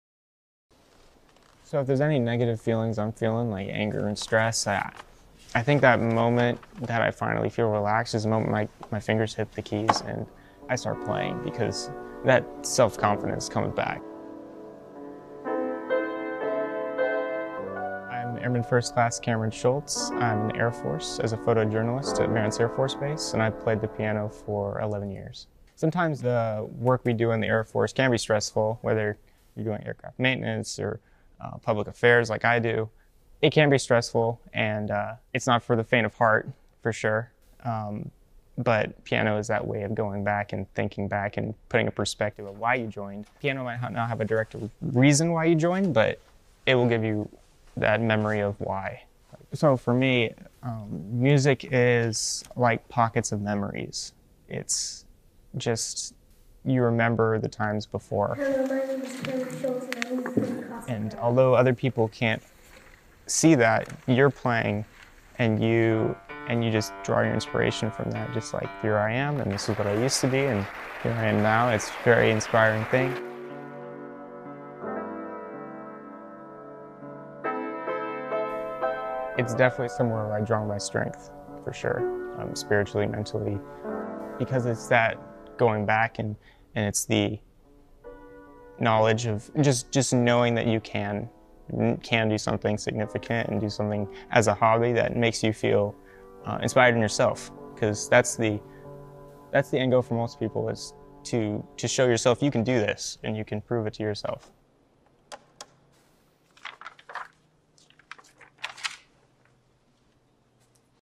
More than Music (2019) 🇺🇸
🔴 [Author]:
Video by Tech. Sgt. Erik Cardenas

🔴 [Unit]:
71st Flying Training Wing Public Affairs

🔴 [Location]:
Enid, Ok, United States

🔴 [Description]:
A1C Cameron Schultz is a member with the 25th Flying Training Wing, Public Affairs Office at Vance Air Force Base. He is a photojournalist and on his off-duty time plays the piano. (U.S. Air Force video by Tech. Sgt. Erik Cardenas)

🔴 [Tags]:
military, feature, aetc, piano, upt

🔴 [VIRIN]:
191205-F-YM869-935